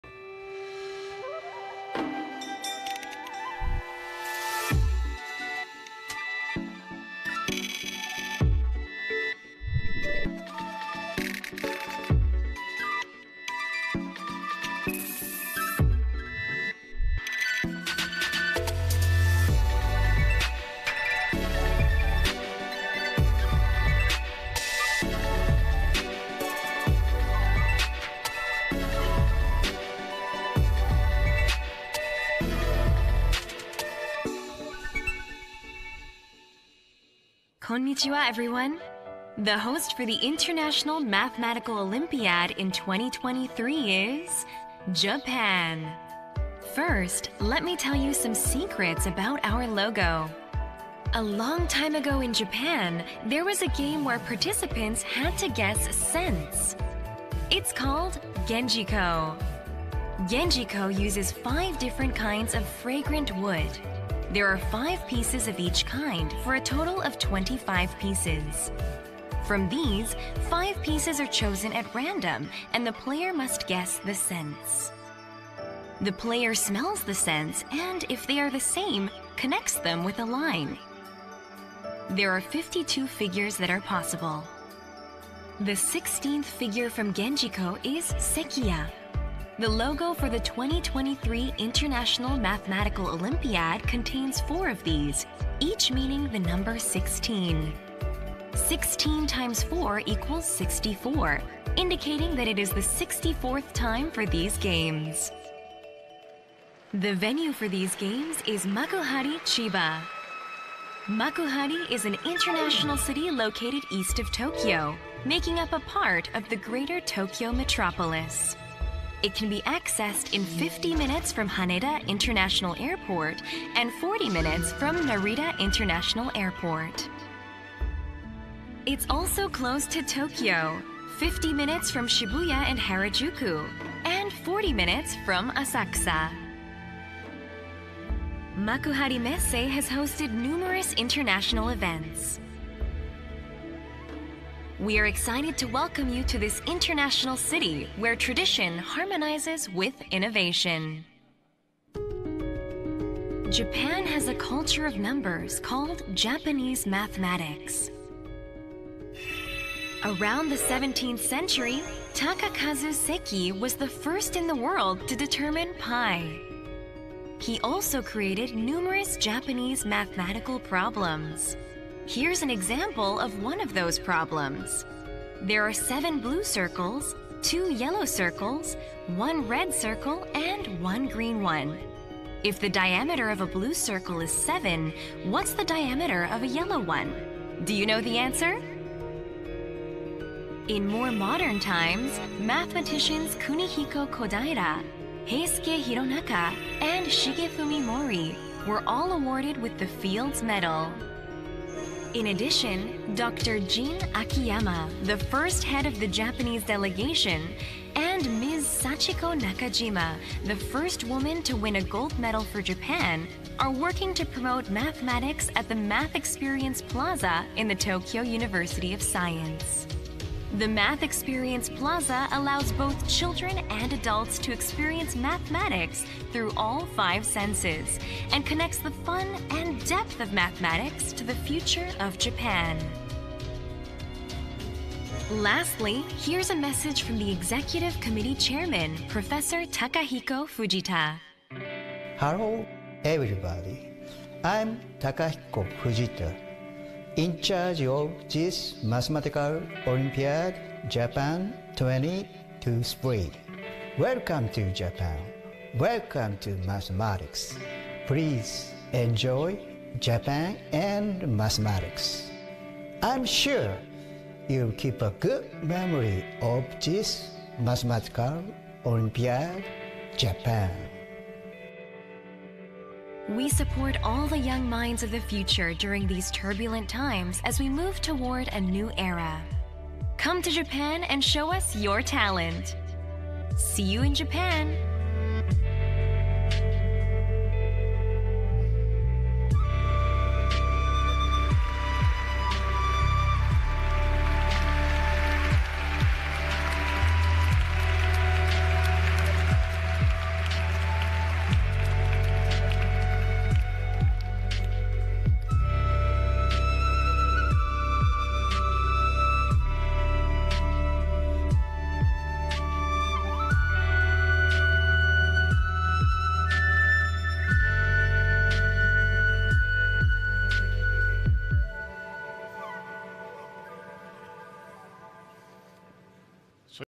第64届IMO宣传片/Promotional video of the 64th IMO
第64届国际数学奥林匹克竞赛将于2023年在日本千叶举行。

The 64th International Mathematical Olympiad will be held in 2023 in Chiba, Japan.